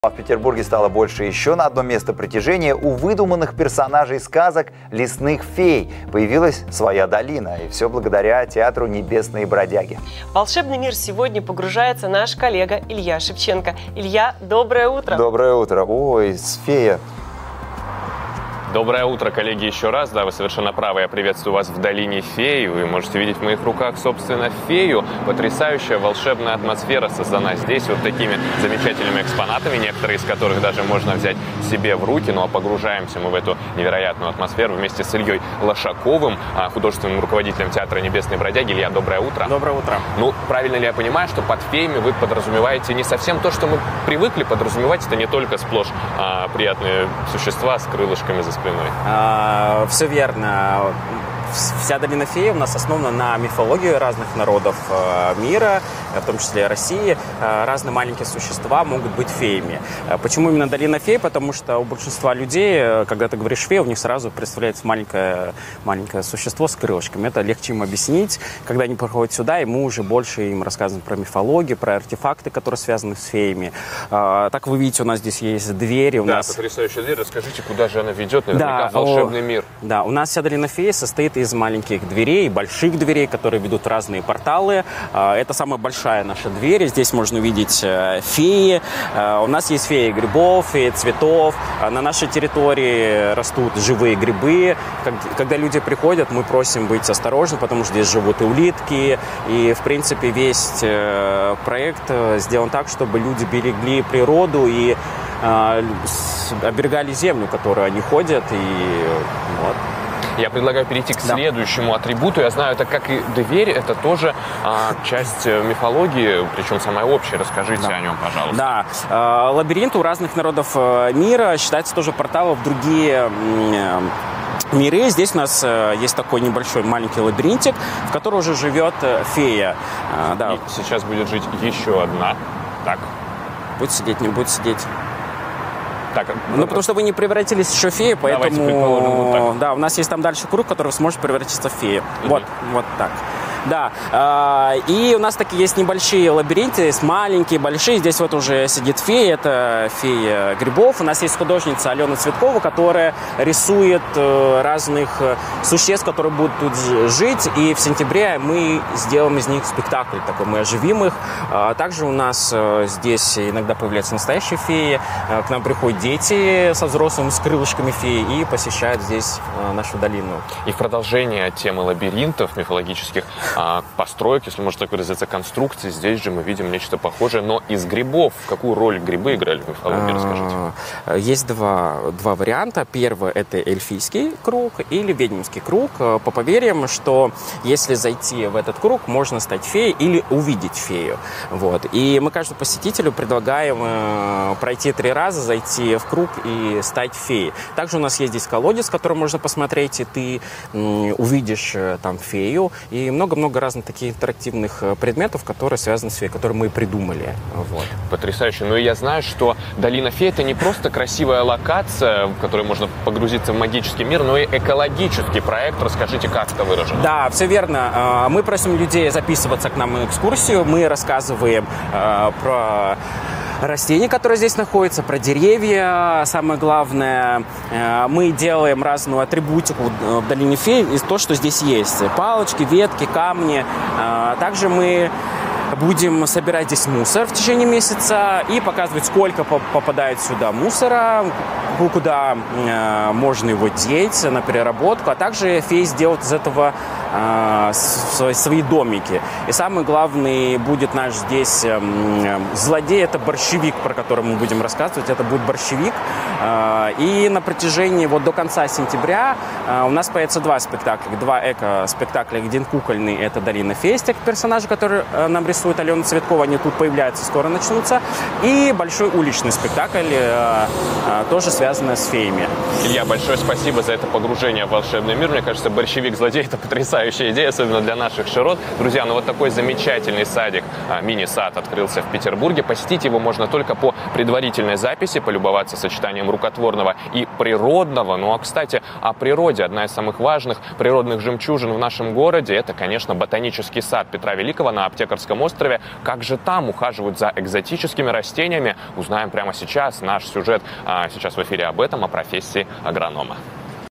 Долина фей в Петербурге: мир волшебных лесных существ
В Петербурге стало больше еще на одно место притяжения. У выдуманных персонажей сказок - лесных фей - появилась своя Долина. Всё благодаря театру «Небесные Бродяги». В волшебный мир погрузился Илья Шевченко